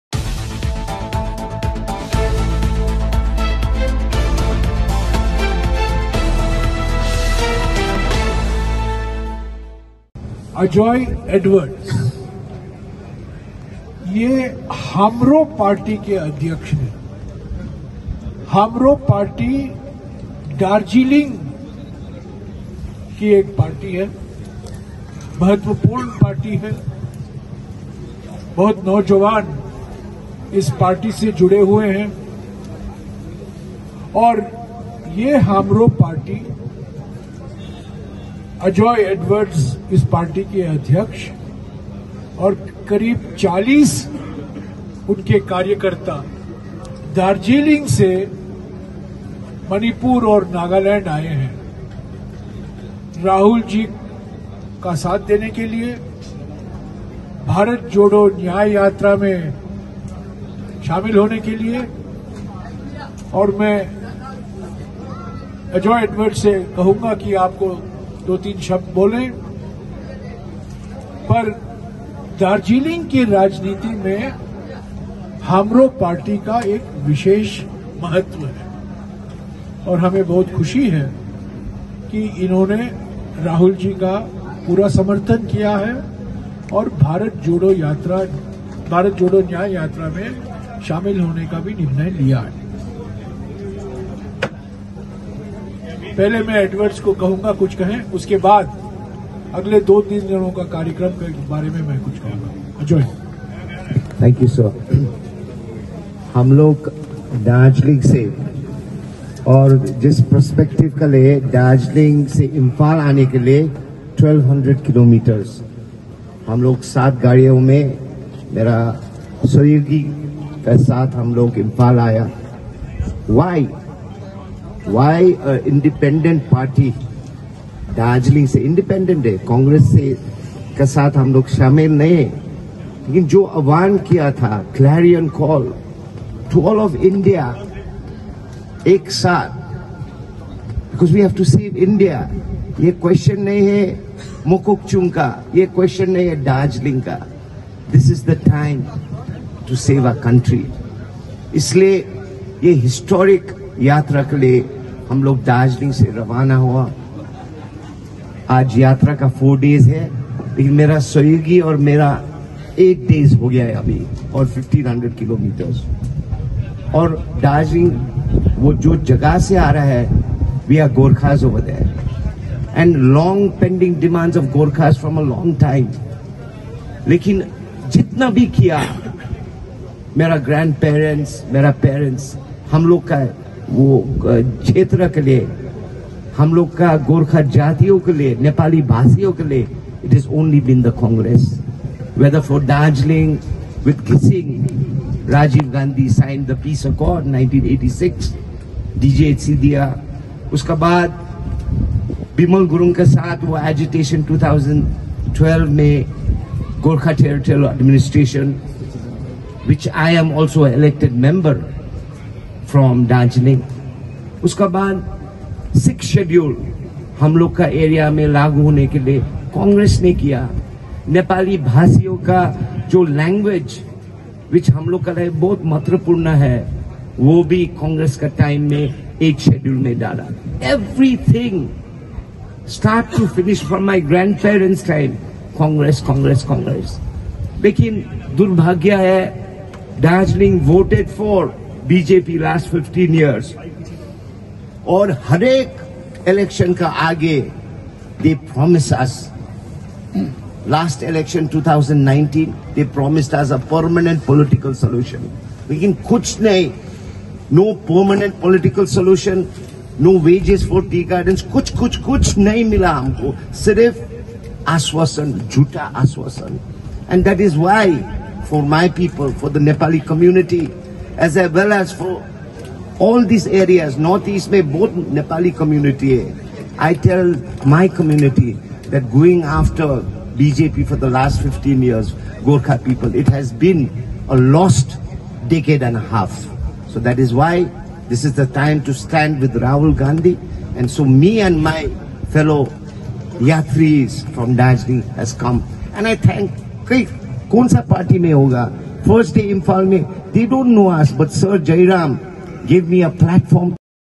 दार्जीलिङ पहाड अनि गोर्खाहरूले आजसम्म जे जति पाए काग्रेसकै शासनकालमा नै पाएका छन : एडवाडर्स
जयराम रमेशले भने दार्जीलिङको राजीतिमा हाम्रो पार्टीको अहम भूमिका छ।

दार्जीलिङ 17 जनवरी: दार्जीलिङ पहाड अनि गोर्खा हरूले आजसम्म जे जति पाए काग्रेस सरकारको शासनकालमा नै पाएका छन यो मनतब्य हाम्रो पार्टी अध्यक्ष अजय एडवाडर्सले पोखेको हो।कागे्रसको भारत जोडो न्याय यात्राले आज नागाल्याण्ड राज्यको विभिन्न जिल्लाहरूको भ्रमण गरयो। भारत जोडो न्याय यात्राको नेतृत्व काग्रेस संसद राहुल गाँधीले गरेका छन। न्याय यात्रामा काग्रेसको शीर्ष नेतृत्वहरू पनि सामेल रहेका छन। भारत जोडो न्याय यात्राको क्रममा काग्रेसको बरिष्ठ नेता एंव पूर्व केन्द्रीय मंत्री जयराम रमेशले एक पत्रकार सम्मेलन डाकेर हाम्रो पार्टी अध्यक्ष अजय एडवाडर्स अनि उनको टोली दार्जीलिङबाट भारत जोडो न्याय यात्रामा सामेल हुन आएका छन भन्ने बताए। मणिपूरको इम्फालदेखि शुरू भएको भारत जोडो न्याय यात्रा आज नागाल्याण्ड राज्य आई पूगका छन अनि अजय एडवाडर्सहरू पनि राहुल गाँधीसंग संगै न्याय यात्रामा सामेल भएका छन। दार्जीलिङको राजनैतिक पृष्ठ भूमिमा हाम्रो पार्टीको अहम भूमिका रहेको कागे्रसी कार्यकर्ता एंव पूर्व केन्द्रीय मंत्री जयराम रमेशले बताउँदै हाम्रो पार्टी अध्यक्ष अजय एडवाडर्सलाई आयोजित पत्रकार सम्मेलनमा आफ्नो पक्ष राख्ने अनुरोध गरयो।आयोजित पत्रकार सम्मेलनलाई सम्बोधन गर्दै हाम्रो पार्टी अध्यक्ष अजय एडवाडर्सले भारत जोडो न्याय यात्रामा सामेल हुन हामी दार्जीलिङबाट आएका हौ,हाम्रो पार्टीको काग्रेससंग कुनै सम्बंध छैन तर काग्रेस पार्टीले भारत जोडो न्याय यात्राको जो आहवान गरेको छ, त्यसदेखि हामी अति नै प्रवाहित बनेका छौ। आसम, मणिपूर अनि दार्जीलिङ मात्र होइन आज भारत देशभरिको अवस्था चिन्ता जनक रहेको छ। भारत जोडो न्याय यात्रामा हामी सामेल भएको आज चैथो दिन हो। हाम्रो पार्टी अध्यक्ष अजय एडवाडर्सले अधि भने भारतीय जनता पार्टीले दार्जीलिङको गोर्खाहरूको दीर्धकालिन मांगलाई पूरा गर्ने वाचाहरू गर्दै भोट हत्याउँने कार्यहरू मात्र गरेको छ गर्दै आजसम्म गोर्खाहरूको दीर्धकालिन मांग पूरा भएको छैन तिनले भने। हाम्रो पार्टी अध्यक्ष अजय एडवाडर्सले अधि भने दार्जीलिङ पहाड अनि गोर्खाहरूले आजसम्म जे जति पाएको छ केवल काग्रेसको शासनकालमा नै पाएको तिनले बताउँदै सन 1986 को गोर्खाल्याण्ड आन्दोलनपछि 1988 सालमा दार्जीलिङ गोर्खा पार्वतीय परिषदको दस्तावेजमाथि त्रिपक्षीय सम्झौता भएको थियो, त्यस समय राजीव गाँधी प्रधानमंत्री थिए। त्यसरी नै 2007 मा विमल गुरूङले 2007 मा गोर्खा जनमुक्ति मोर्चाको गठन गरेर आन्दोलन शुरू गरेका थिए अनि 2011 मा केन्द्रको कागे्रस गठबन्धन सरकारले गोर्खाल्याण्ड क्षेत्रिया प्रशासनको दस्तावेजमाथि त्रिपक्षीय सम्झौता गरेको हो। सन 1992 मा केन्द्रमा काग्रेस गठबन्धनकै सरकार थियो अनि नेपाली भाषाले सम्बैधानिक मान्यता पाएको हो। दार्जीलिङ पहाड अनि गोर्खाहरूले आजसम्म जे जति पाएको छ काग्रेसको शासनकालमा नै पाएको तिनले बताए। विगत 15 बर्षदेखि भारतीय जनता पार्टीले गोर्खा हरूलाई भ्रम जालमा हाल्दै भोट हत्याउँने काम गर्दै आई रहेको छ।भाजपाले आफ्नो संकल्प पत्रमा उलेख गरेको एउटै पनि कुरालाई आजसम्म पूरा गरेको छैन।गत 2019 को लोकसभा चूनावमा दार्जीलिङ पहाड एंव गोर्खाहरूको दीर्धकालिन समस्यालाई स्थायी राजनैतिक समाधान गर्ने लिखित वाचा गरेको थियो तर आजसम्म पूरा गरेको छैन हाम्रो पार्टी अध्यक्ष अजय एडवाडर्सले भने।